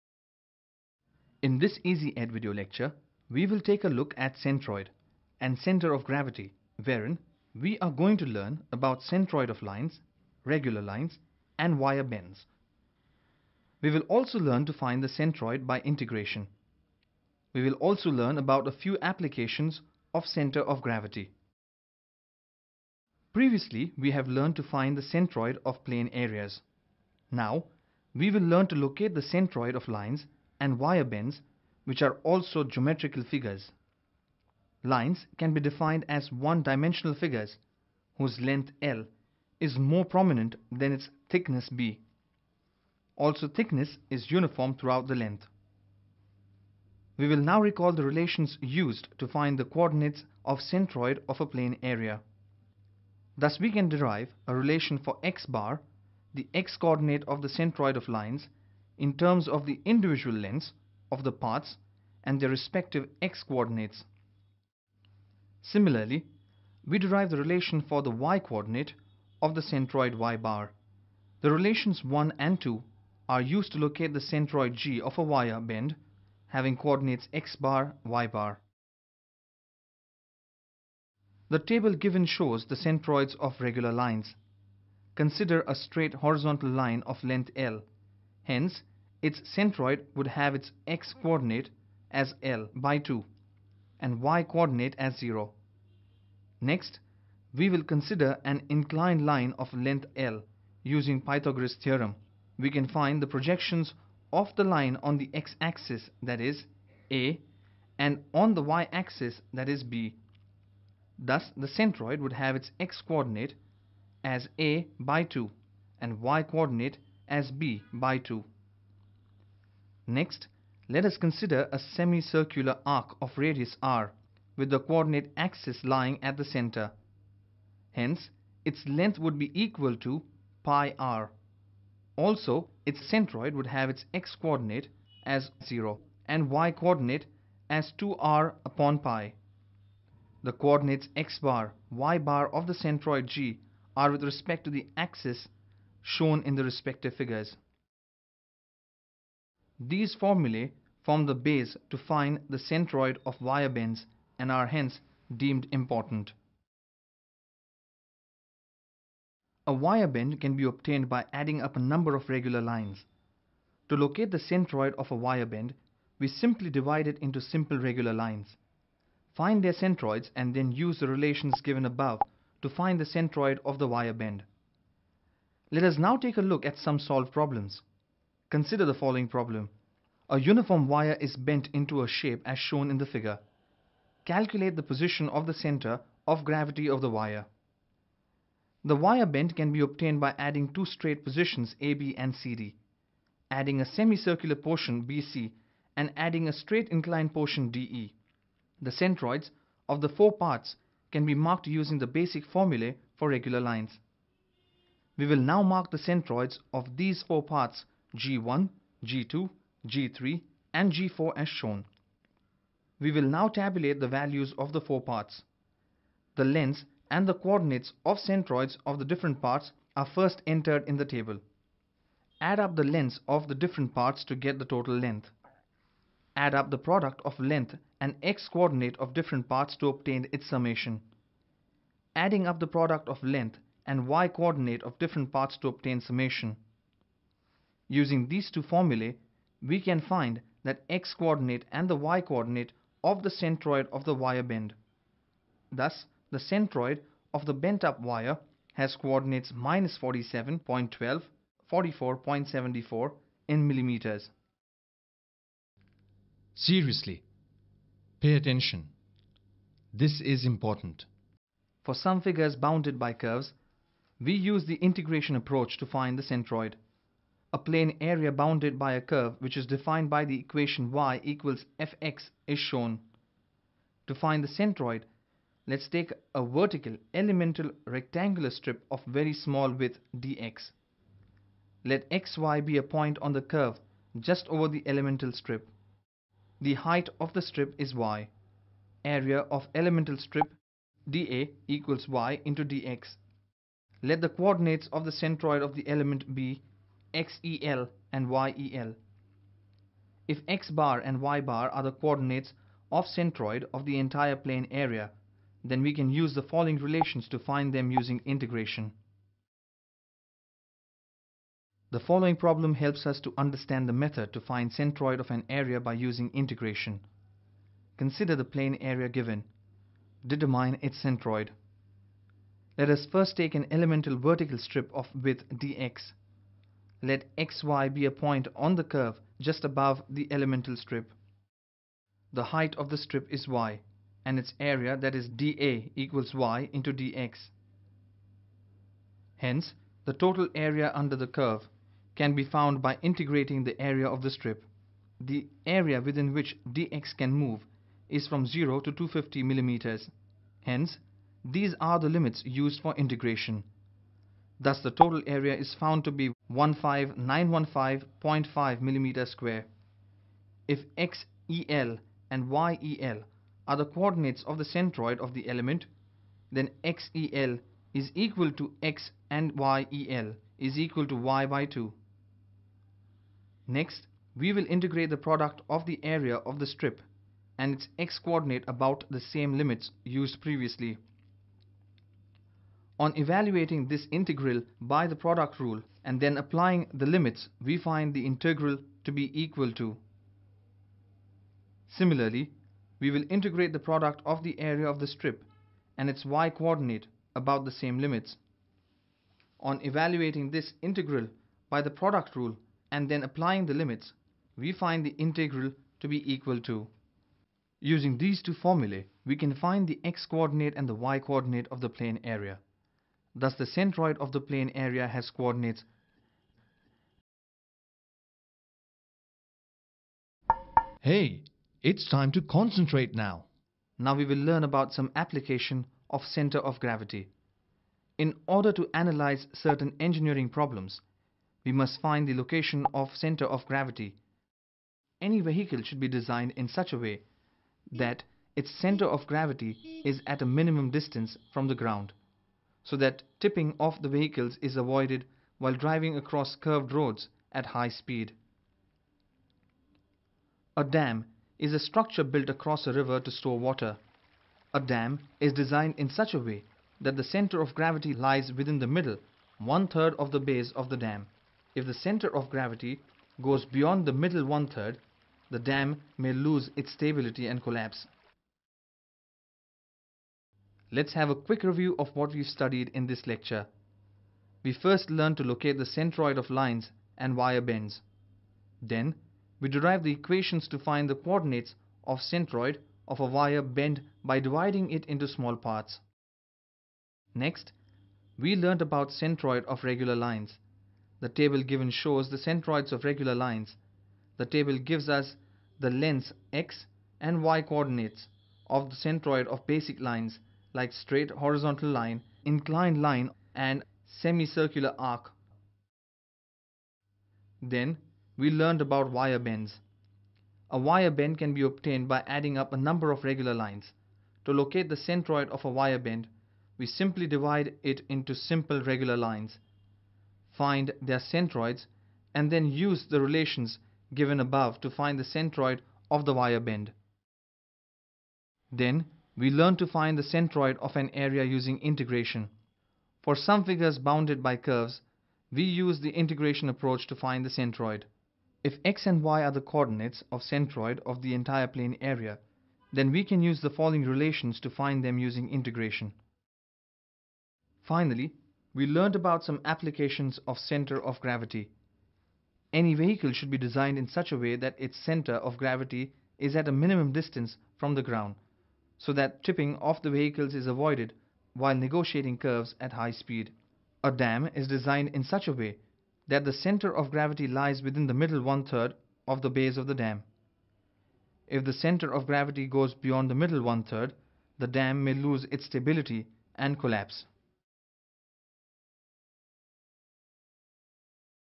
Center Of Gravity And Centroid Part II- Centroid Of Wire Bends - Centroid By Integration
This EzEd Video explains
Center of Gravity and Centroid

- Centroid Of Wire Bends
- Centroid  By Integration